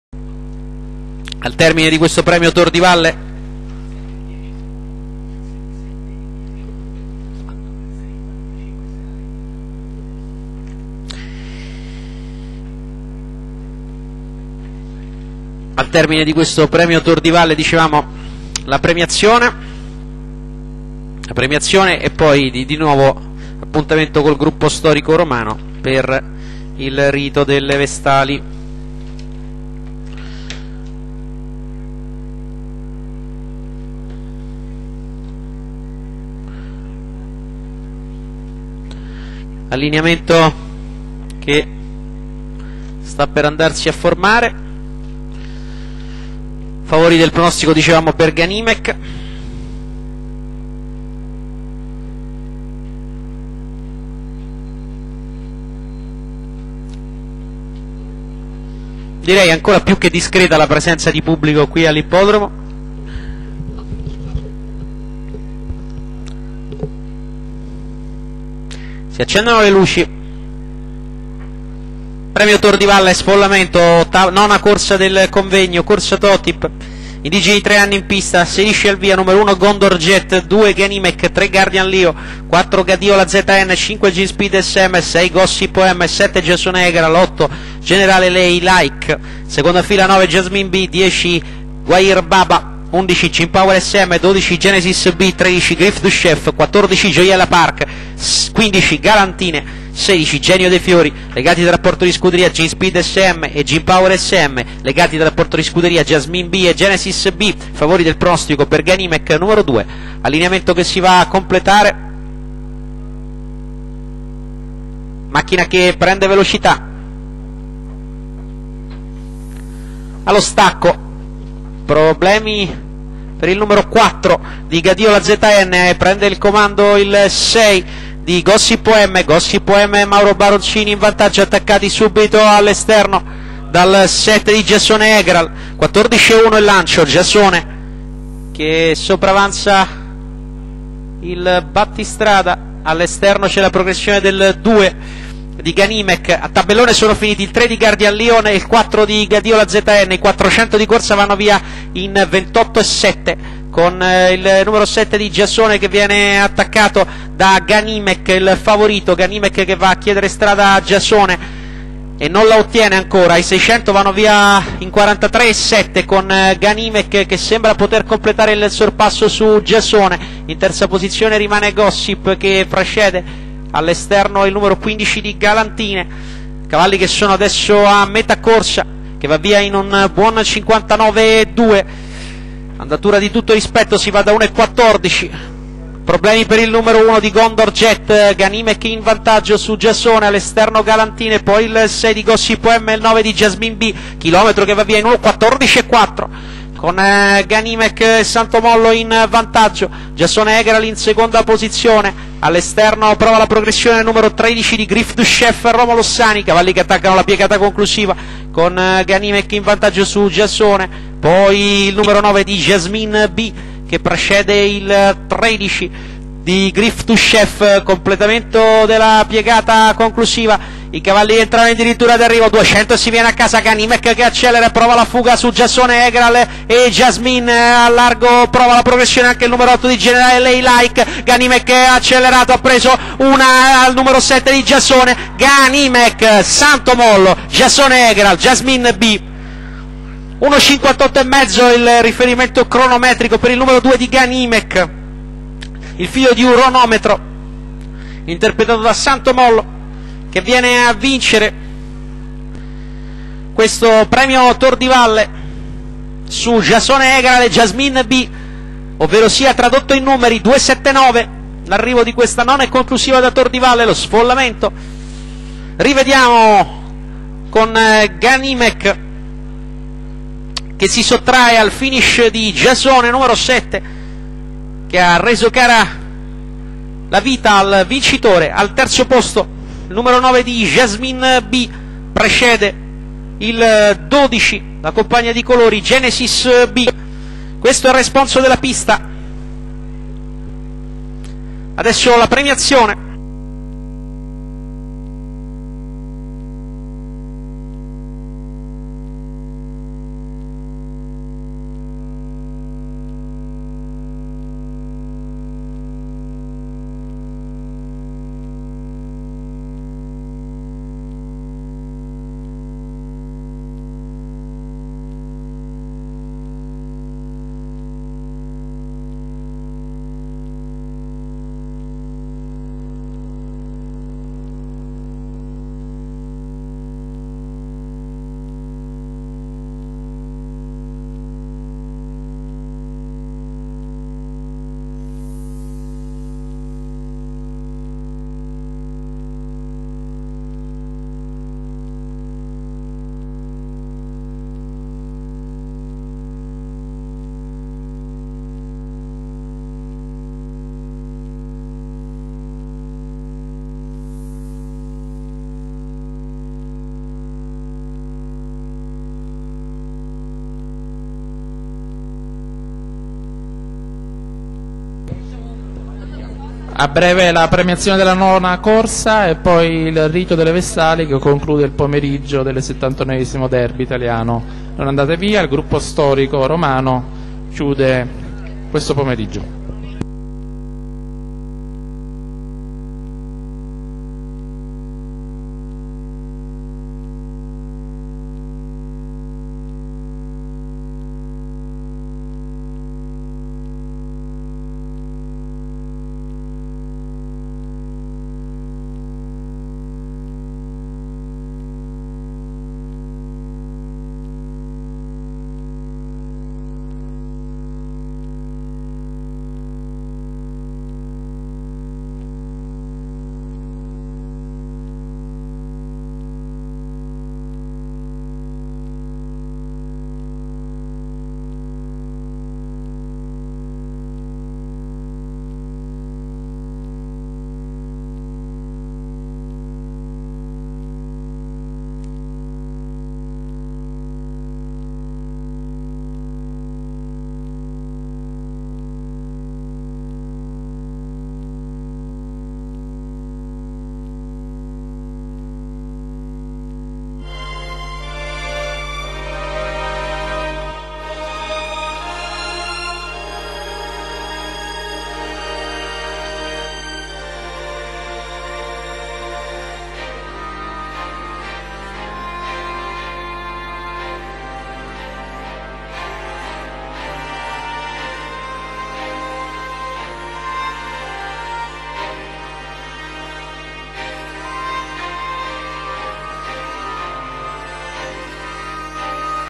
Premio Tor di Valle (Sfollamento Derby) 2006 - GANIMEC
Ganimec vince in 1.14.1 su Giasone Egral e Giasmine Bi.